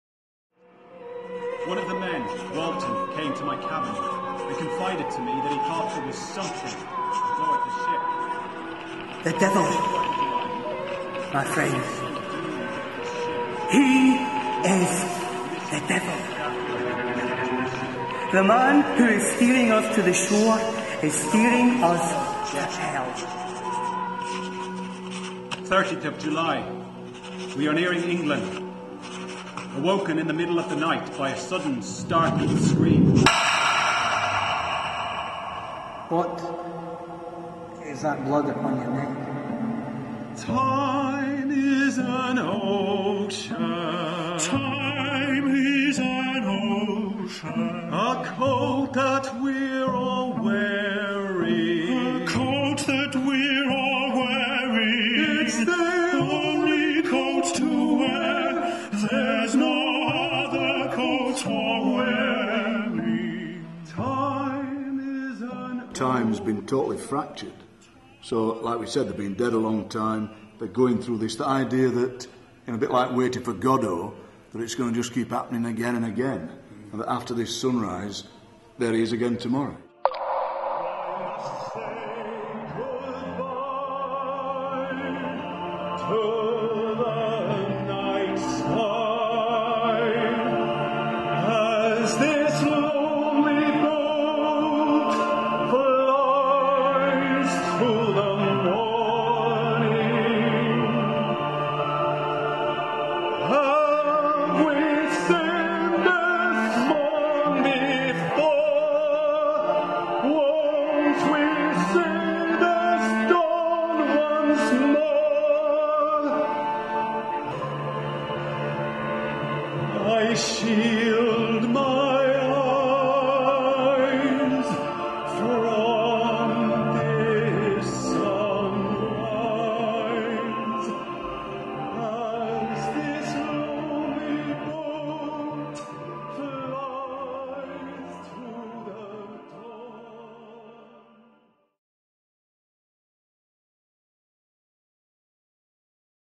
Nosferatu Trailer - Touring Autumn 2015
Trailer for Proper Job Theatre's new production of Nosferatu.
Touring in the Autumn of 2015.
With words/libretto from Ian McMillan and music from from Rod Beale.
The production will be directed by James Beale of Proper Job Theatre Company